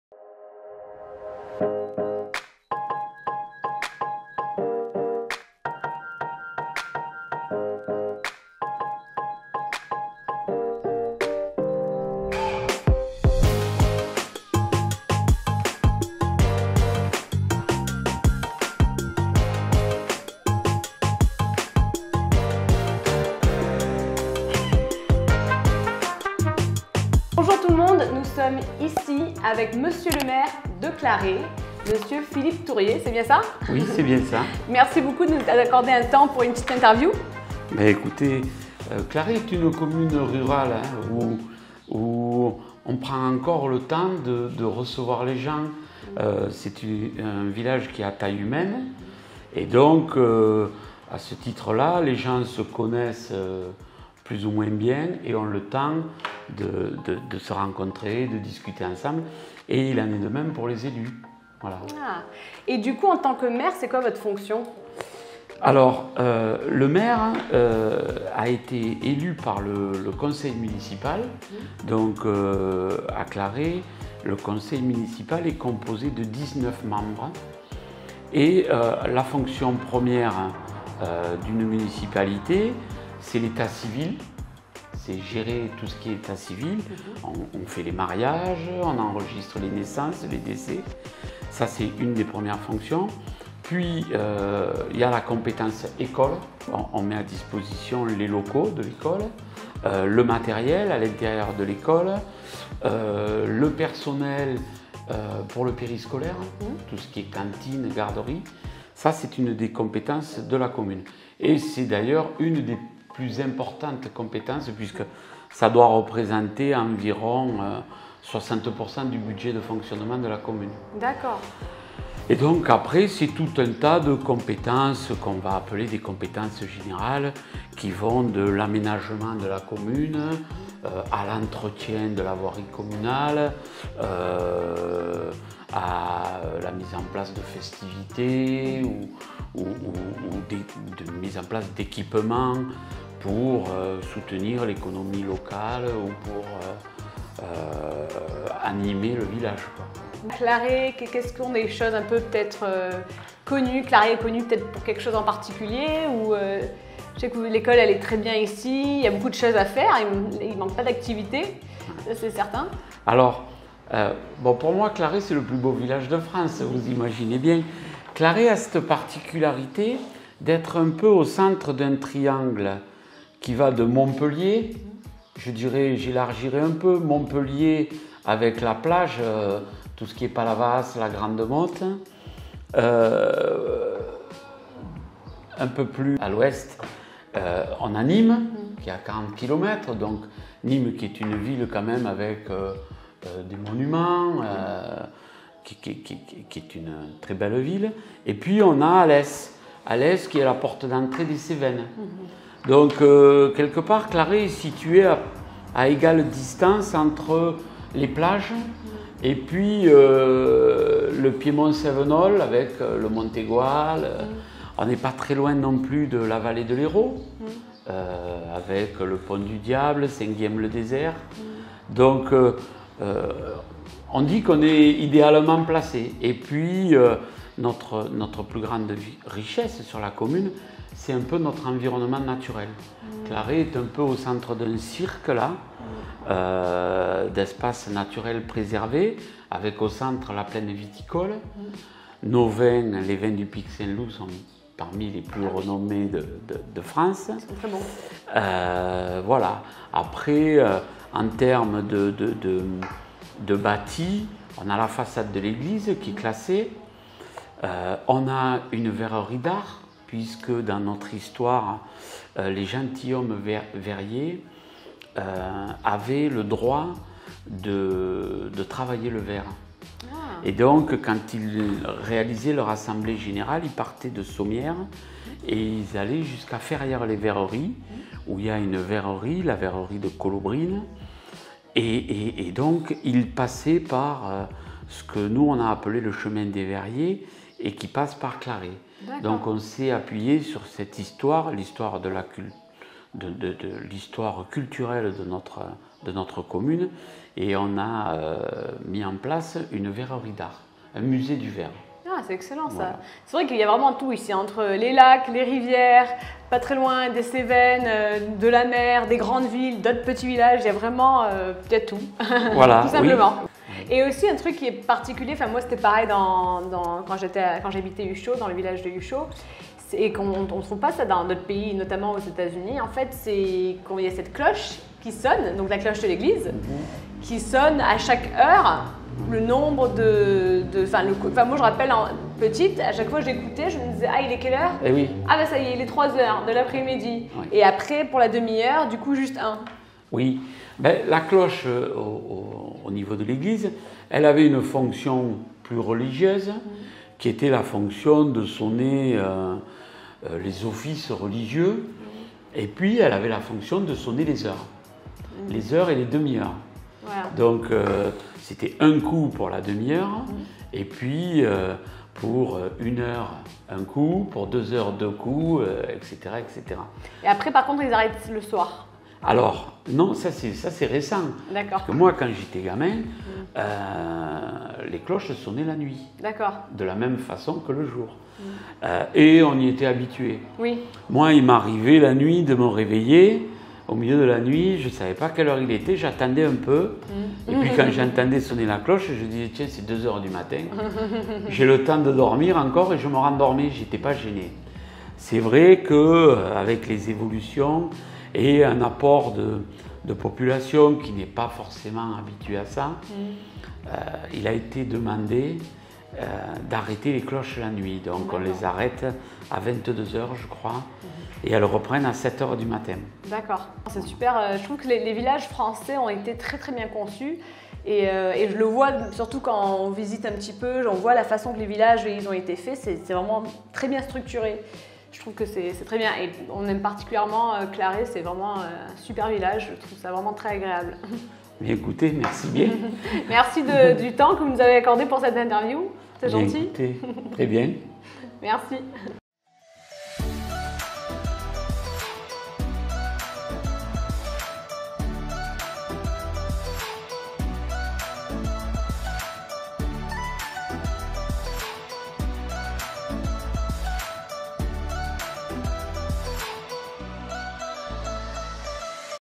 INTERVIEW with a FRENCH MAYOR & WHAT's the ROLE of a MAYOR in FRANCE
Join Rachele DeMeo as we interview the mayor of Claret, in Southern France: Philippe Tourrier. He explains what his job entails, as a French mayor. He explains how a French mayor is voted in. Mr Tourrier explains the different responsibilities he has as a mayor in France. He also explains what's so special about this tiny French town, Claret.

Rejoignez Rachèle DeMéo alors que nous interviewons le maire de Claret, dans le sud de la France: Philippe Tourrier. Il explique ce que sont ses fonctions, en tant que maire français. Il explique comment un maire français est voté en tant que maire. Mr. Tourrier explique ses différentes responsabilités en tant que maire français. Il explique aussi ce qui rend ce petit village de Claret, si spécial.